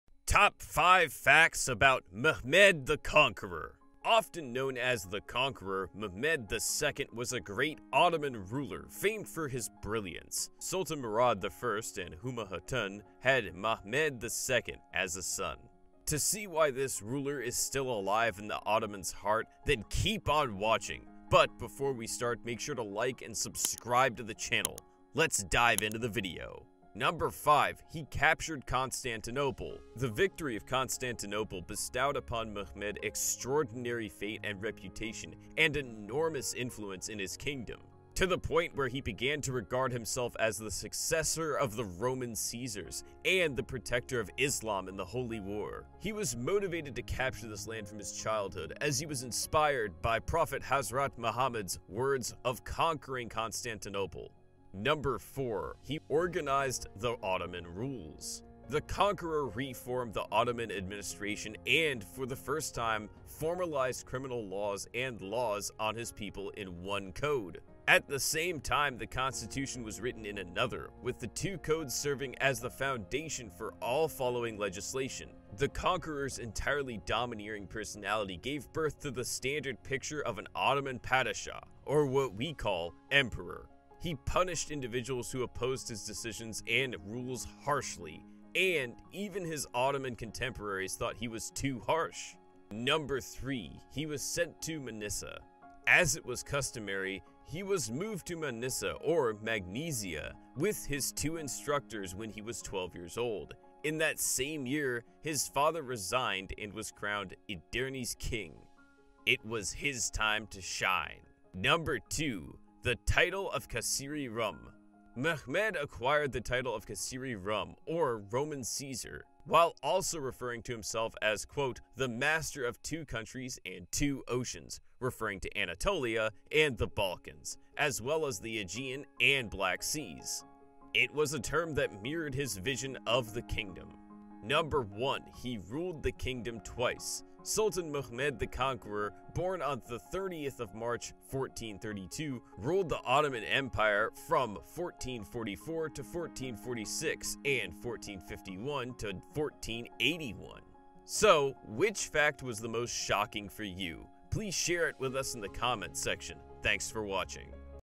Fun Facts about Mehmed The Conquereor
In today's Fun Fact Friday we explore 5 facts about Mehmed the conqueror!